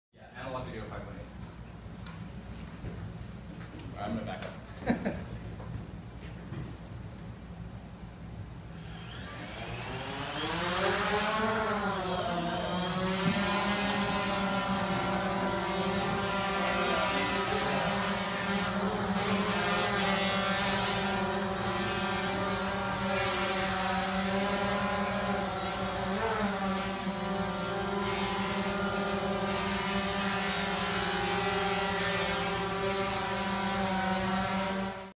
Eric Cheng at dorkbotSF #70
Eric Cheng - Aerial Imaging and Drones: A Different Perspective
dorkbotSF #70
10/12/13
dimension 7